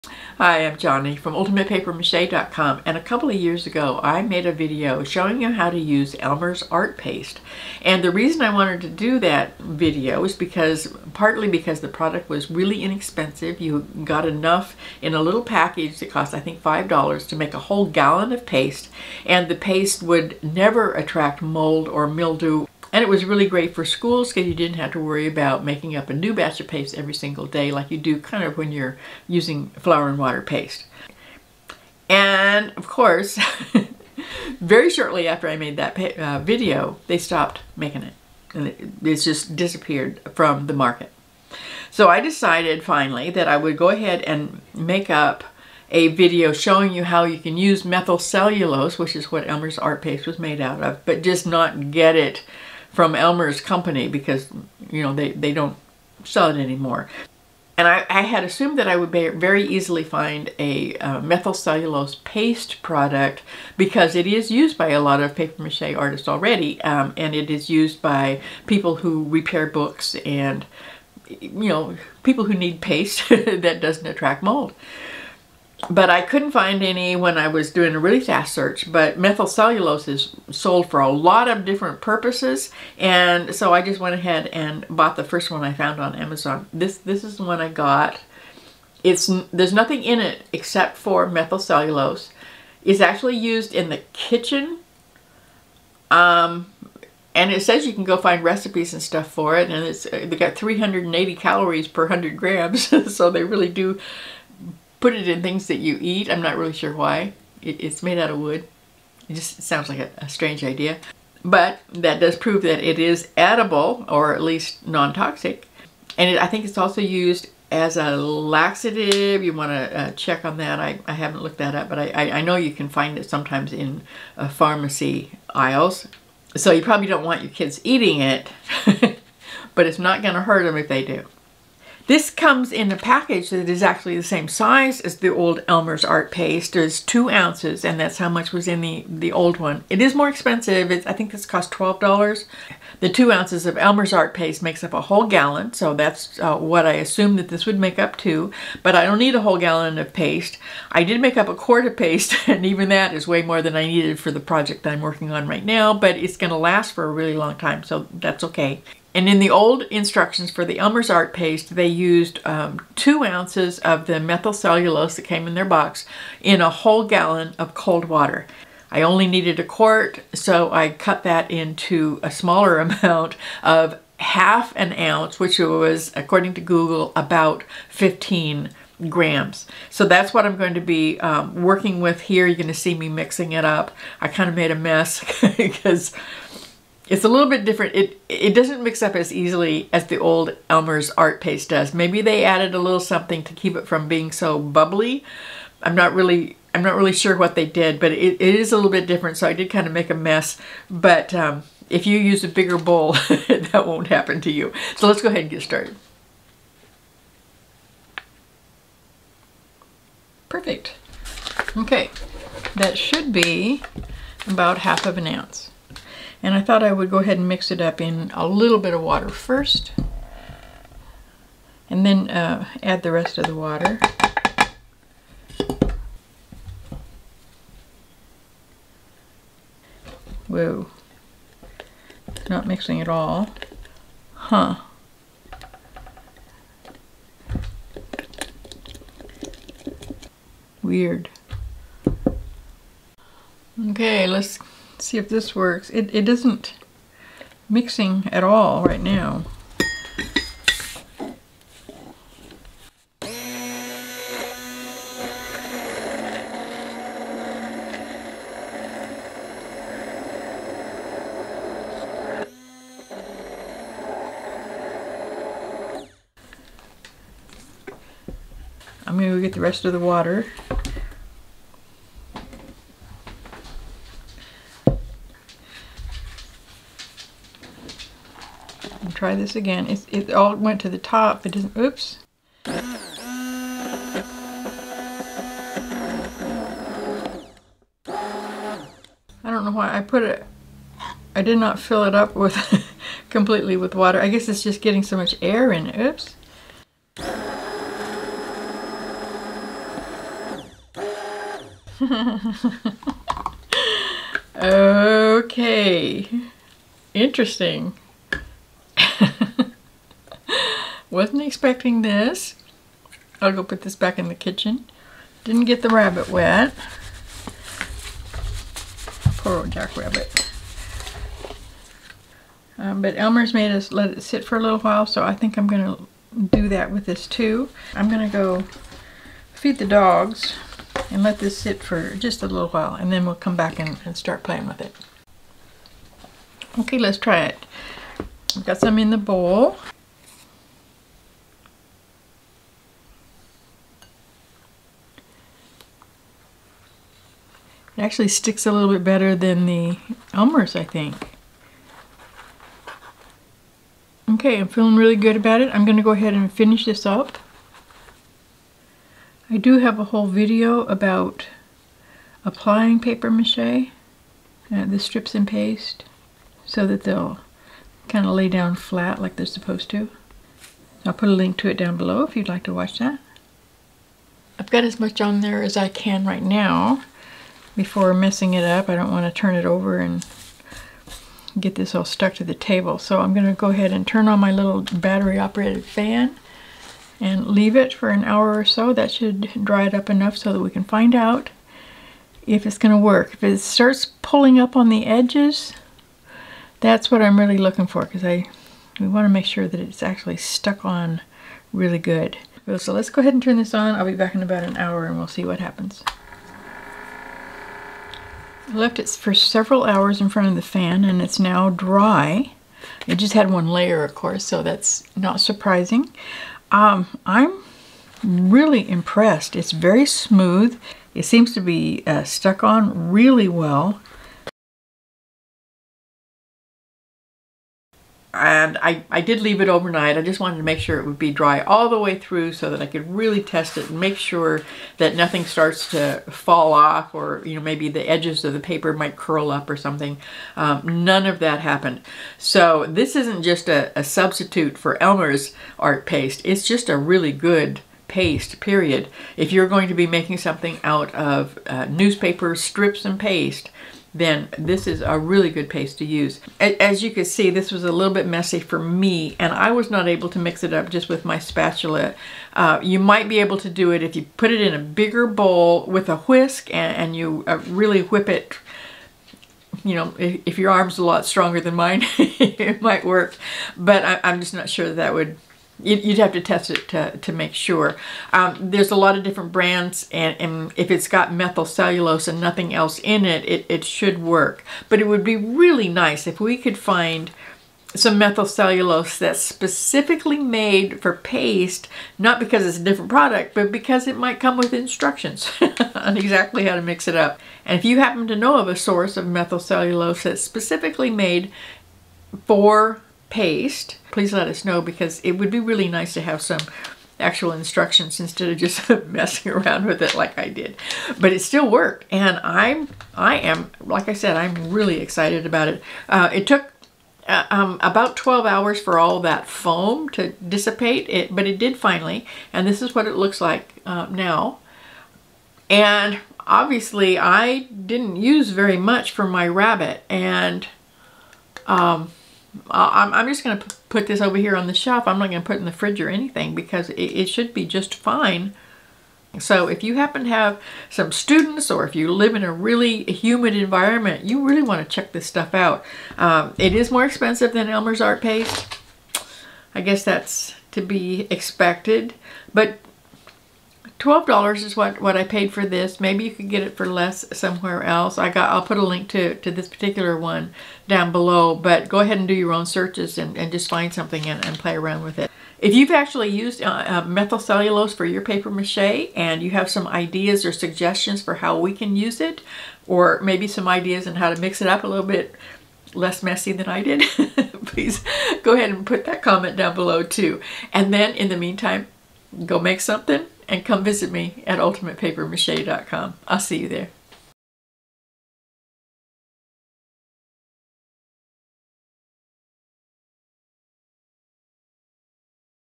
Elmer's Art Paste Alternative - Pure Methylcellulose
If you've looked for Elmer's Art Paste recently, you know they don't sell it anymore. But there is an alternative that works even better - and like the Art Paste, it doesn't attract mold or mildew.

Pure Methylcellulose is available for a lot of different uses. I couldn't find a version that was specifically made for paste, so it didn't come with instructions for mixing it. I just used the same method for mixing that came with the old Elmer's Art Paste, using 2 oz in one gallon of cold water (or 1/2 oz in a quart of cold water).